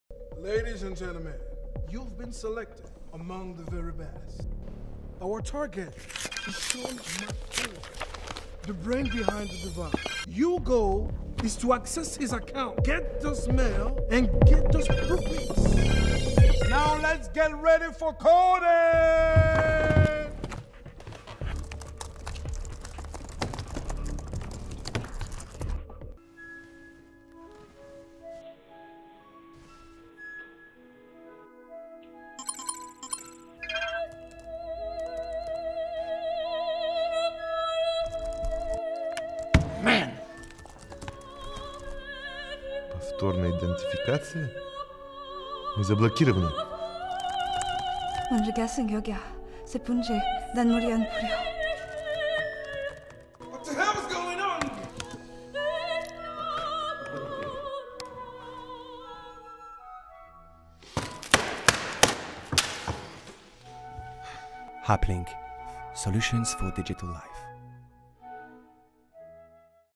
The FIDO Security Key commercial on Bmpcc
Commercial shot for the Fido Security Key by Happlink.
Shot on a Black Magic Pocket Cinema Camera.

Shot on Black Magic Pocket Cinema Camera. Edited in Premiere Pro.

Directed and edited by Jonathan Pineau Bonetti